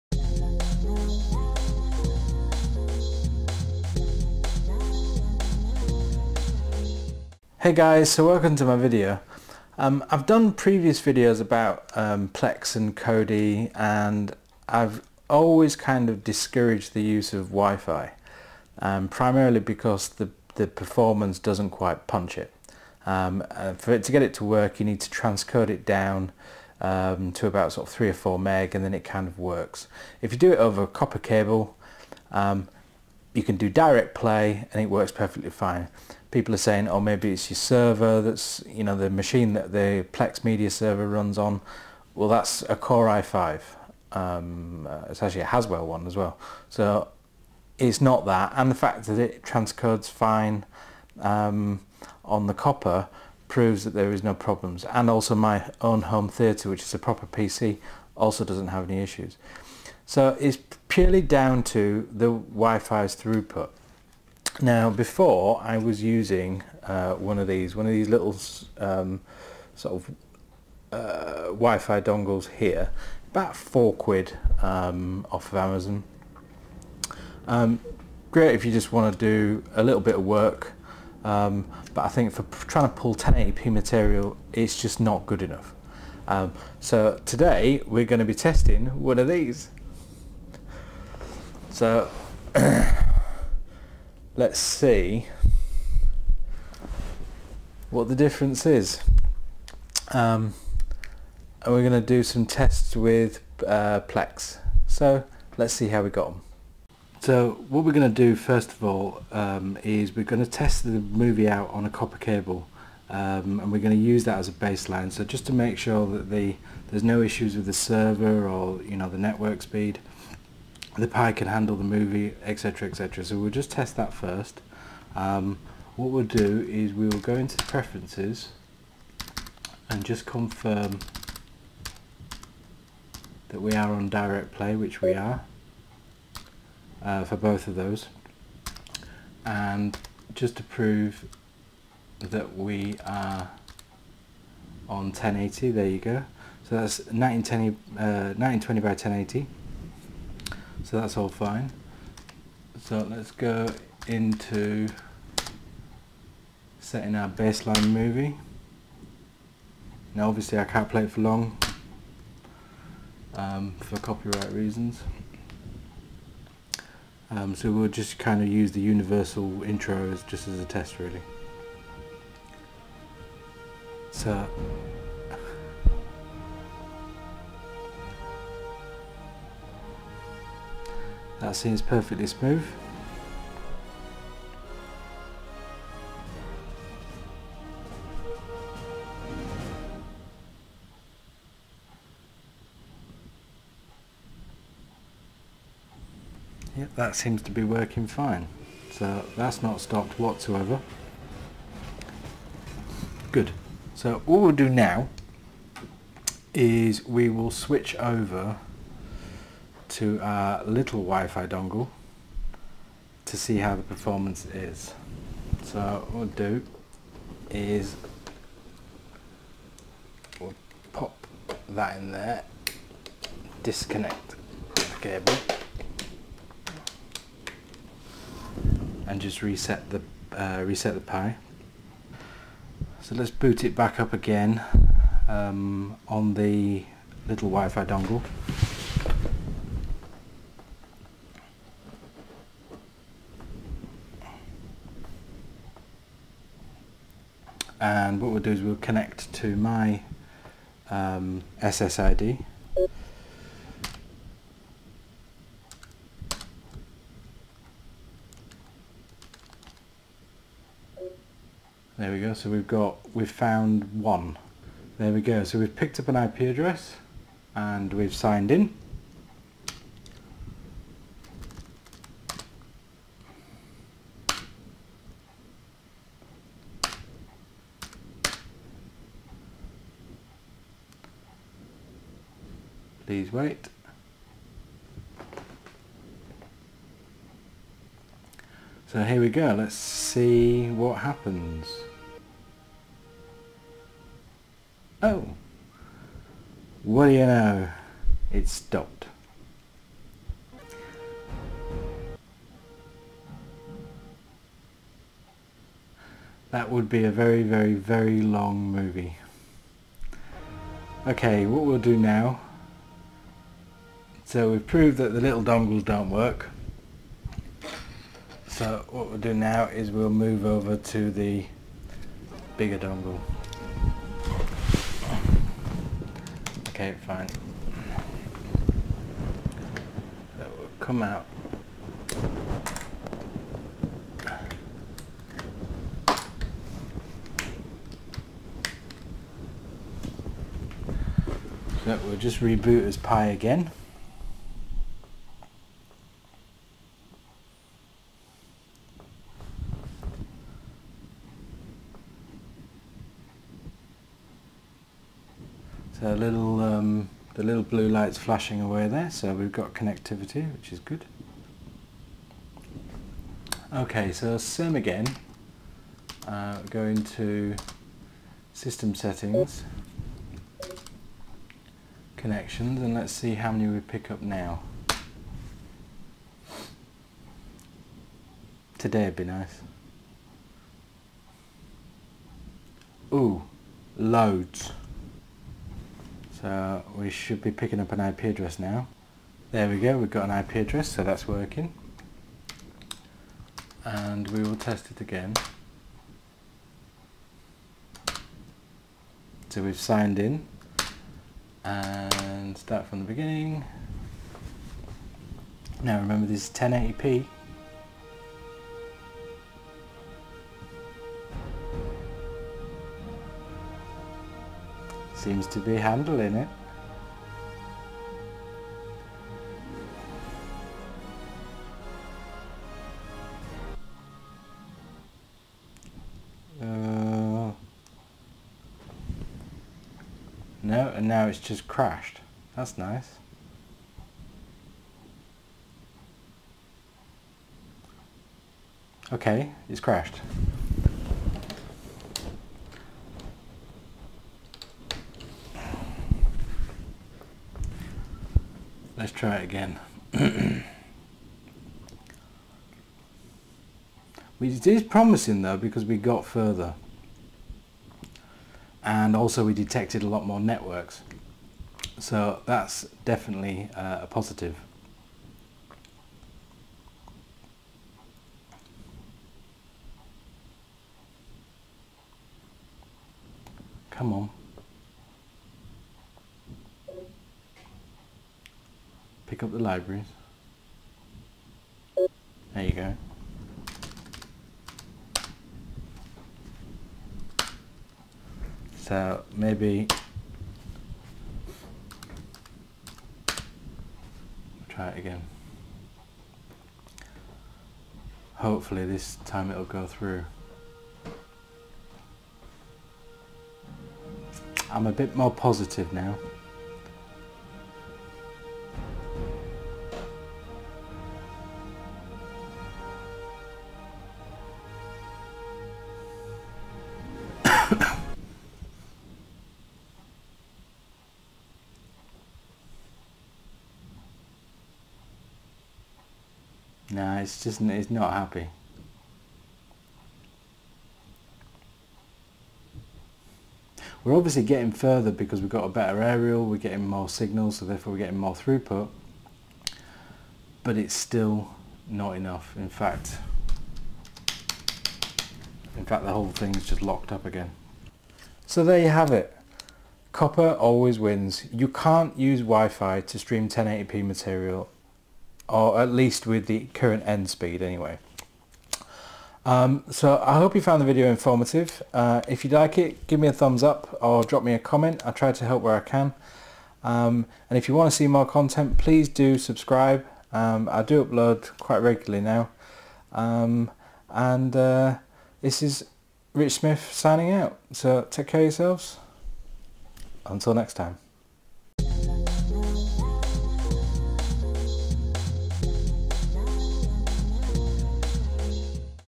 RPi2 : PLEX Wireless Dongle Showdown vs LAN cable on 1080p
Many people stated in my previous videos that my wireless dongle was too poor to stream 1080p material.  So i dug out a proper dongle with a huge aerial.

Here I test some 1080p material on

LAN Cable
Tiny Compact Dongle
Dongle with Large Aerial

guess this video is aimed at people thinking they can stream full bit rate 1080p.

720 or 720...upscaled material to 1080p...ie low bit rate like TV shows works OK.